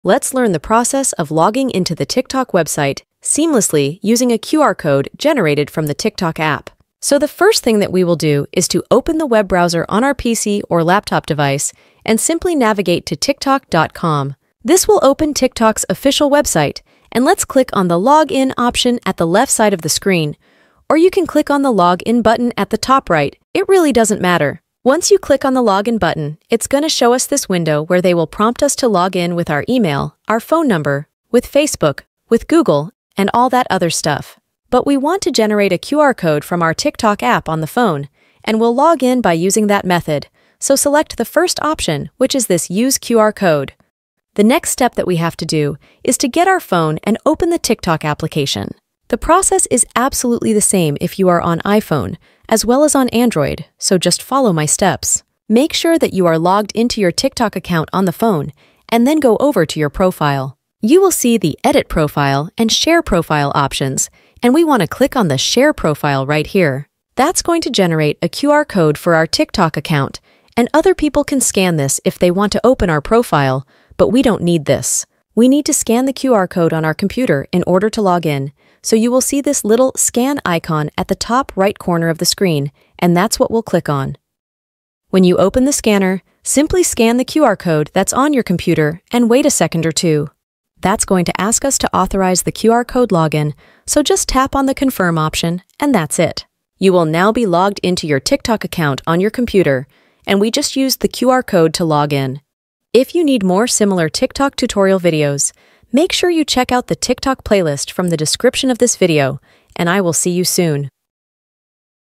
How to Login to TikTok Website Using QR Code From TikTok App (2025)
In this step-by-step tutorial, learn how to effortlessly login to the TikTok website using a QR code generated from the TikTok app itself! As TikTok continues to evolve, so do its features, making it easier than ever to seamlessly access your account across platforms.